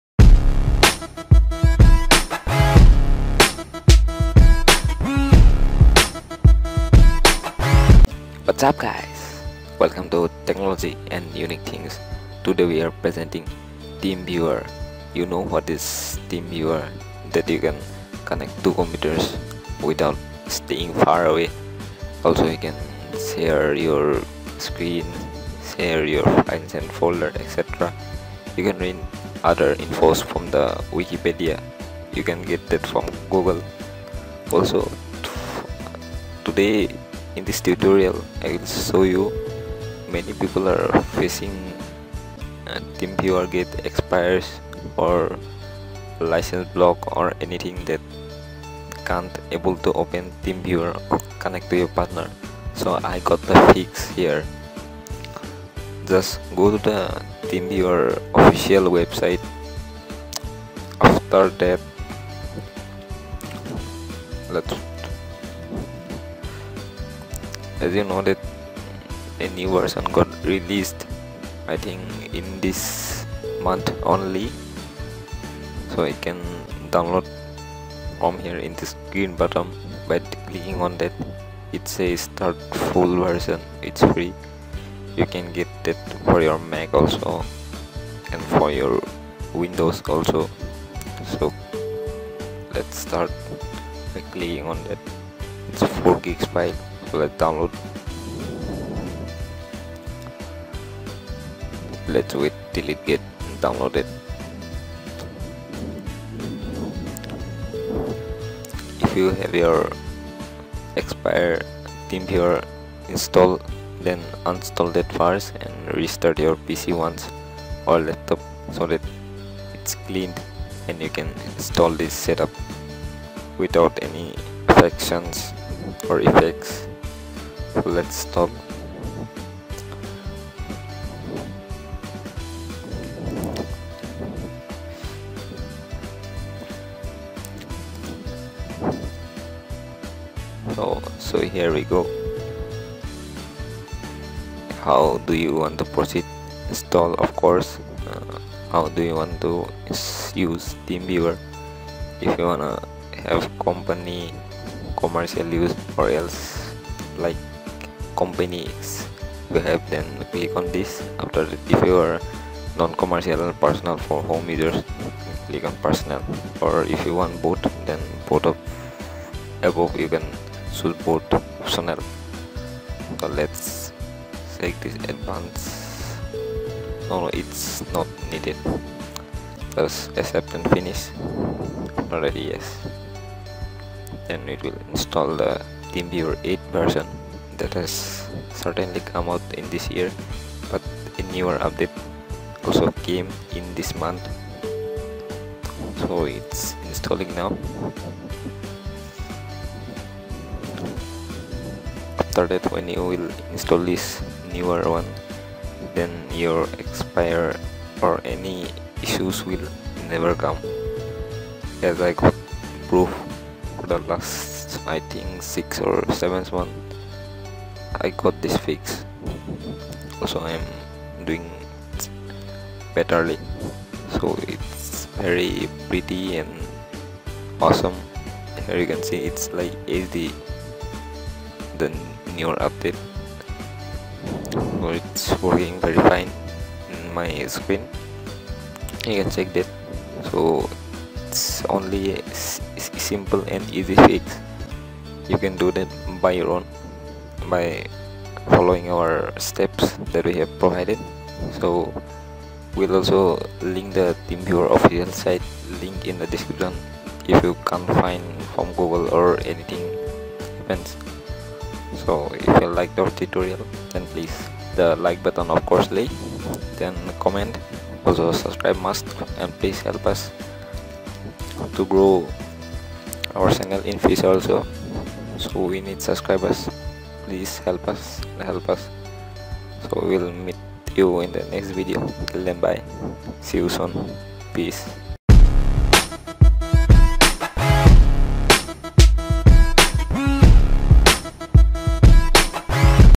Teamviwer Licence Expires Or Any Issue Related Got The Fix
★ Techonology and Unique Things ★
★ Teamviwer Licence Expires Or Any Issue Releated Got The Fix
Helo,Everyone Today I am Presenting The Most Awesome
Software That Teamviwer Fix of Licence Expires Issues and
Related To This You Can Check My Tutorial and Check us Fix
If you have any thing Comment Us Below
Also if you liked it then Please and Please